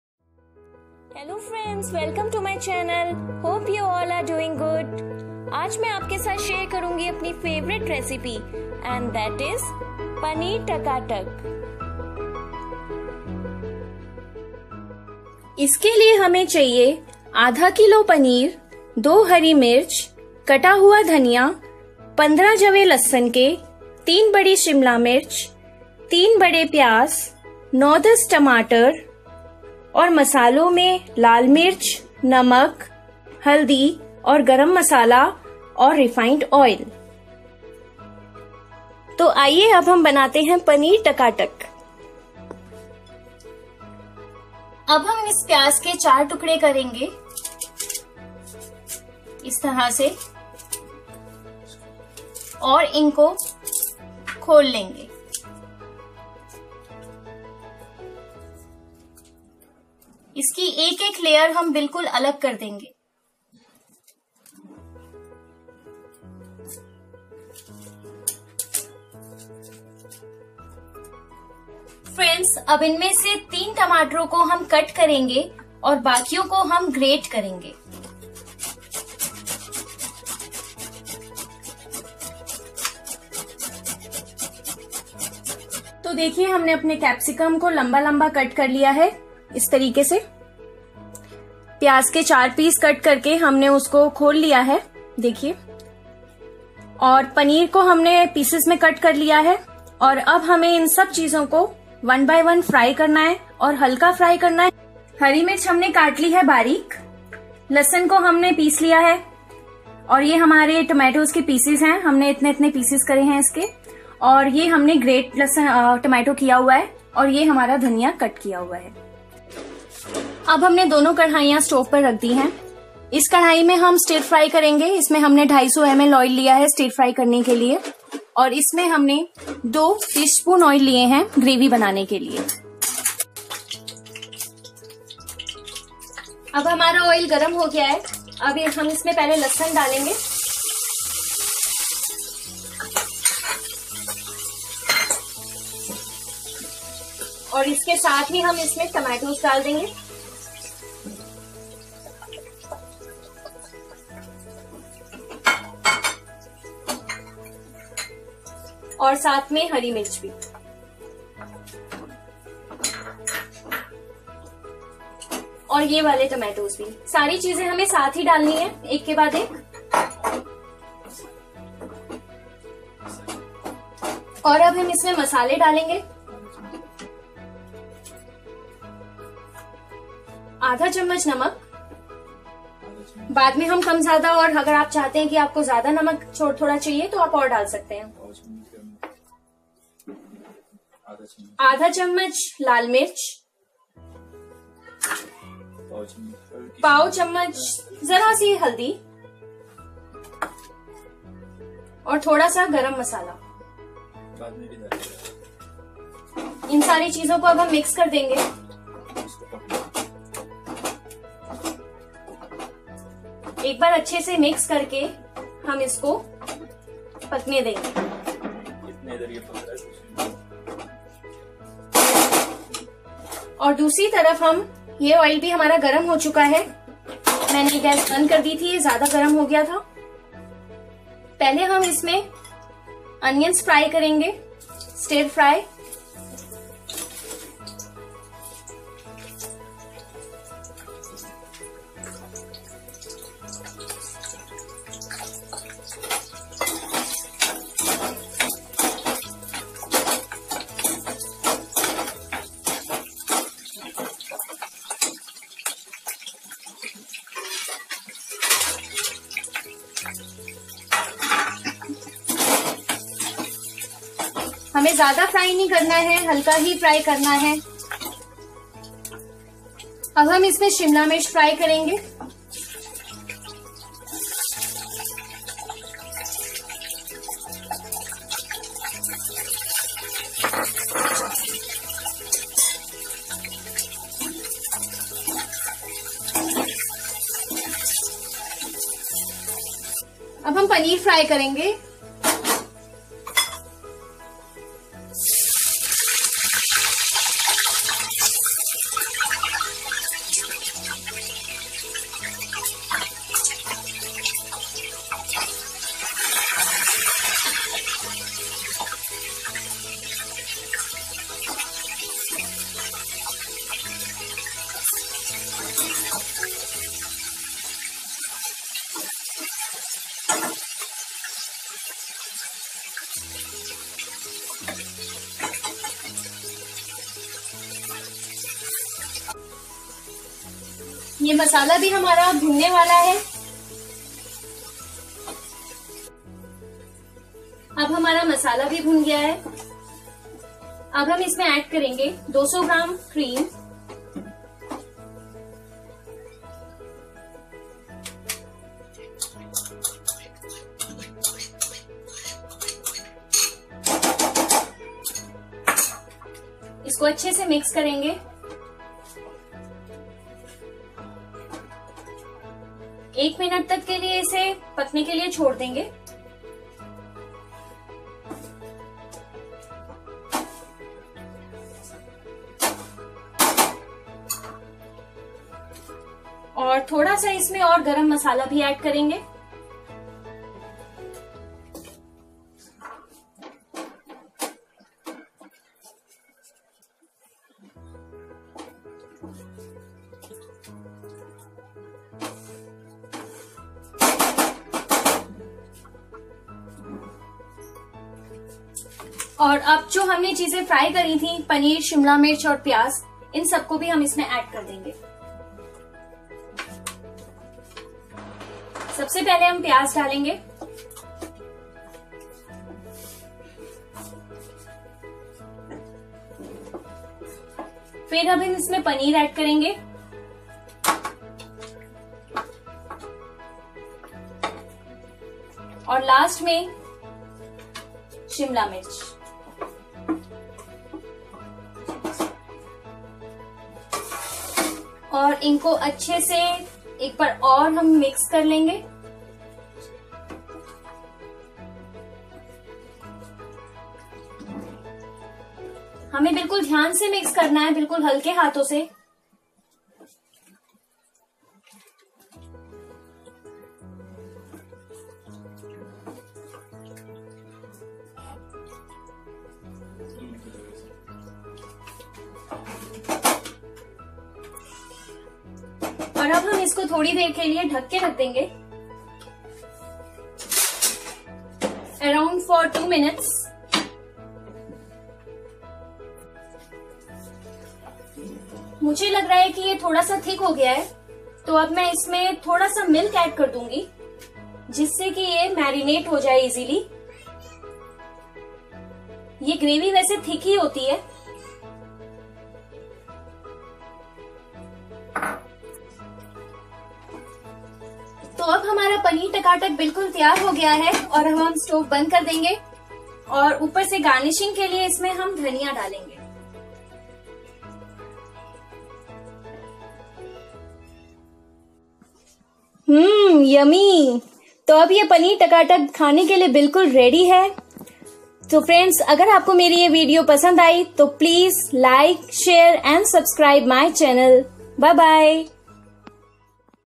paneer takatak recipe | Restaurant style recipe| पनीर टकाटक रेसिपी
Paneer has always been my favorite since my childhood and continues to be. This recipe is easy to make with basic ingredients easily found in your kitchen.
Ingredients
500 gms paneer(cottage cheese)
2 finely chopped green mirch
8 – 10 tomatoes
3 onion
3 capsicum
1/2 tsp chilli powder
1/4 tsp turmeric powder(haldi)
garam masala
10 – 15 cloves garlic
4 tbsp oil
Salt to taste
chopped coriander(dhaniya)
Please hit the  the bell icon beside to be notified whenever I post the video.

LOVE YOU……SEE YOU SOON…..